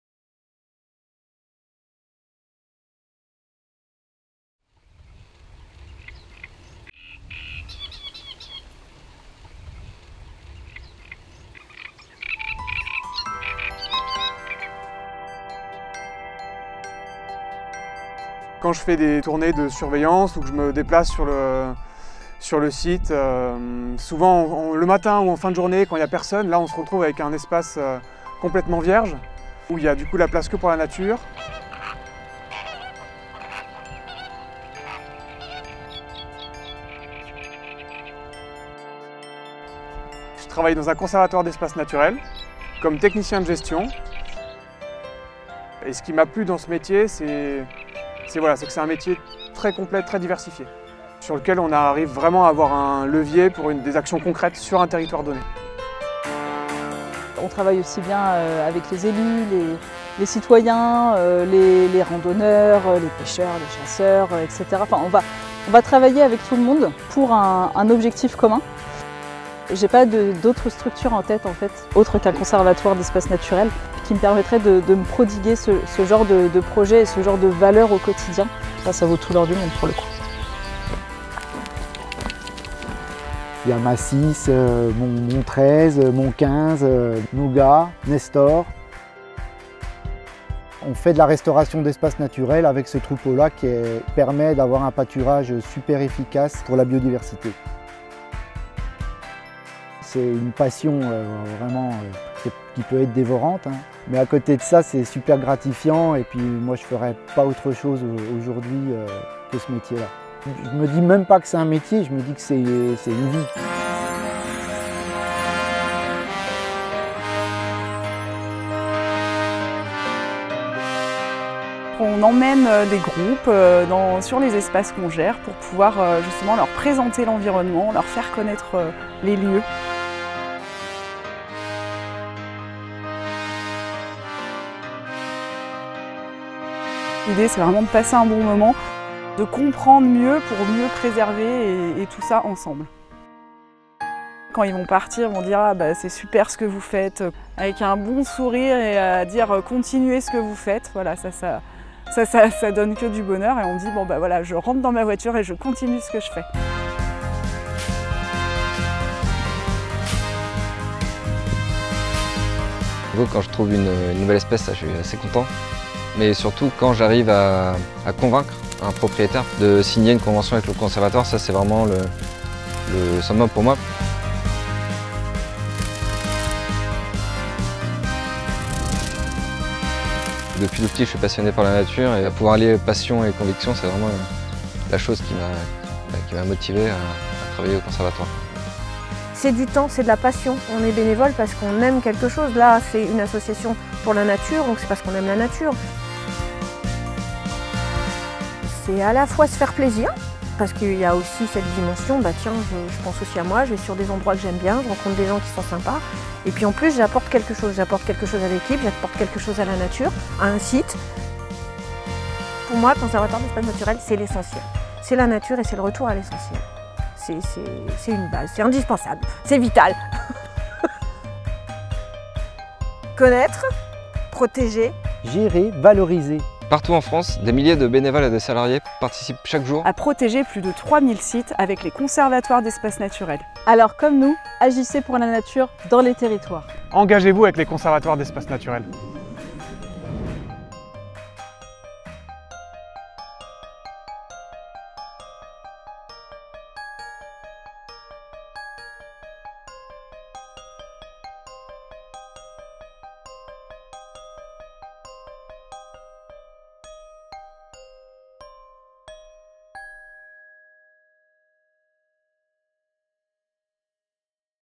Agir pour la nature dans les territoires
Les Conservatoires d’espaces naturels vous présentent leur film-clip « Agir pour la nature dans les territoires » ! Il met en scène les témoignages de six bénévoles et salariés, portés par des paysages exceptionnels au 4 coins de la France. Passionnés, ils œuvrent au quotidien au sein des 3800 sites gérés par les Conservatoires d’espaces naturels.

« Connaître… Protéger… Gérer… Valoriser… Partout en France, des milliers de bénévoles et de salariés participent chaque jour à protéger plus de 3000 sites avec les Conservatoires d’espaces naturels. Alors comme nous agissez pour la nature dans les territoires. Engagez-vous avec les Conservatoires d’espaces naturels ! »

Pour nous soutenir :
-Partager notre film-clip sans modération !
-Faîtes un don pour préserver le patrimoine naturel et la biodiversité avec les Conservatoires d’espaces naturels

Un film de François Nimal

Écrit par Christophe Lépine et Sandrine Poirier

Avec Rémi Jullian (Conservatoire d’espaces naturels d’Occitanie), Caroline Domingues (Conservatoire d’espaces naturels de Normandie), Rémi Vuillemin (Conservatoire d’espaces naturels de Bourgogne), Séverine Willay (Conservatoire d’espaces naturels Rhône-Alpes), Gilles Bailleux (Conservatoire d’espaces naturels de Nouvelle-Aquitaine) & Claire Belin (Conservatoire d’espaces naturels des Hauts-de-France).

Remerciements à Claude, Eliot, Marie-Thérèse, Nicolas, Pascal et Samuel, au Conservatoire du littoral, au Département de l’Ain, aux Conservatoires d’espaces naturels de Bourgogne, Hauts-de-France, Normandie, Nouvelle-Aquitaine, Occitanie et Rhône-Alpes… ainsi qu’à tous les bénévoles et salariés de notre belle famille des Conservatoires d’espaces naturels.

Avec le soutien du Ministère de la Transition écologique et du Crédit Coopératif

Ce film est dédié à Marin Toulier

© Conservatoire d’espaces naturels de Normandie / Fédération des Conservatoires d’espaces naturels / 2021